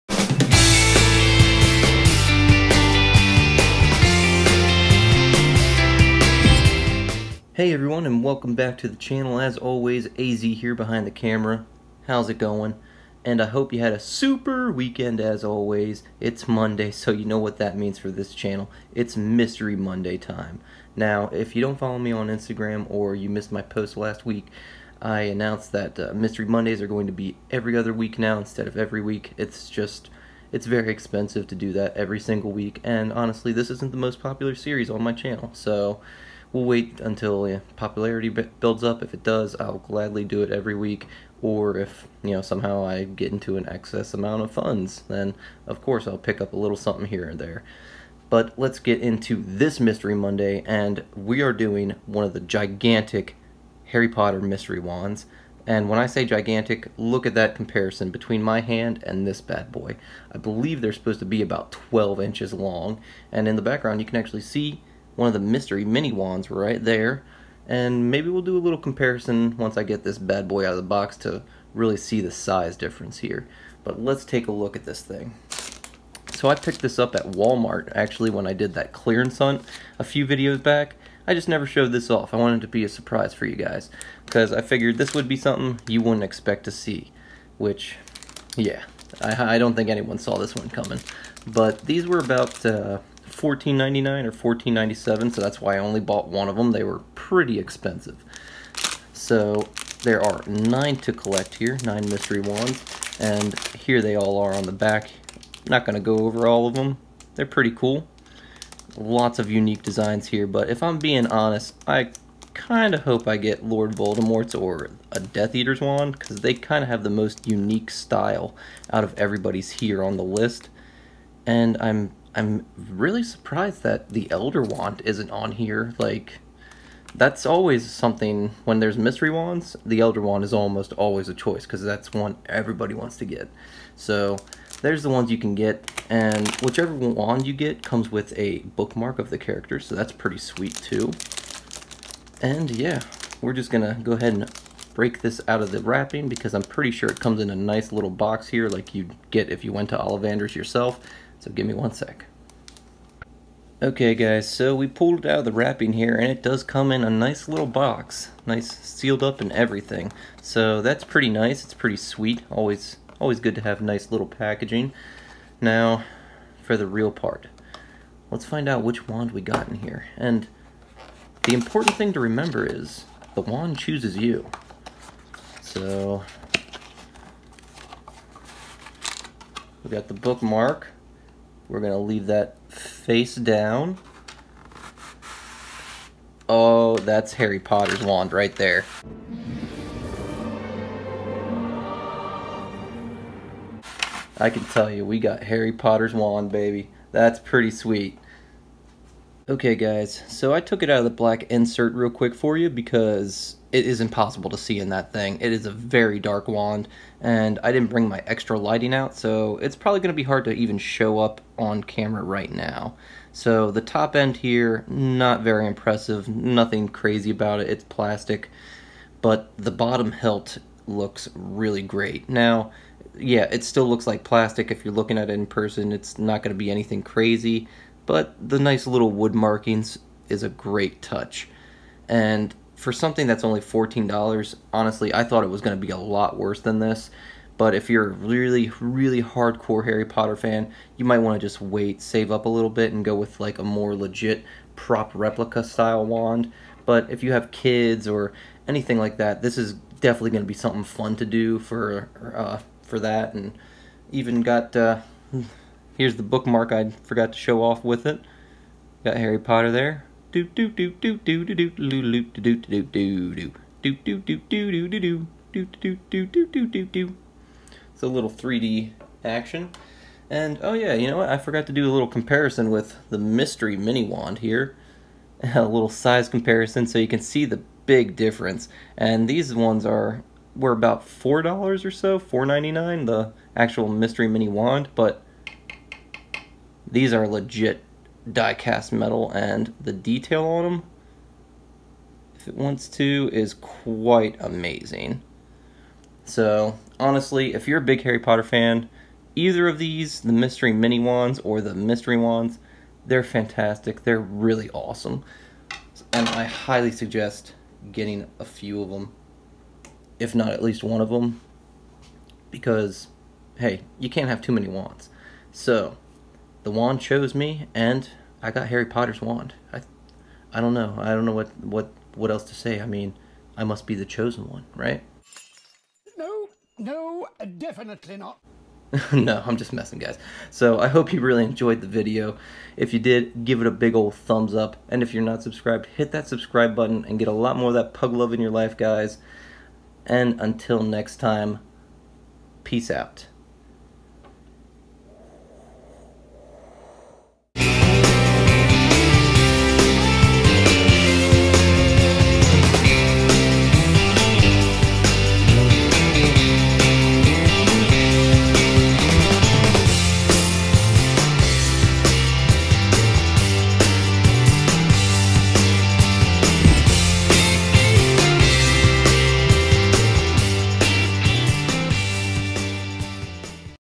Harry Potter 12" Mystery Wand | Mystery Monday #17
In todays Mystery Monday I open up one of the 12" Harry Potter mystery wands. Which wand will choose me? Enjoy!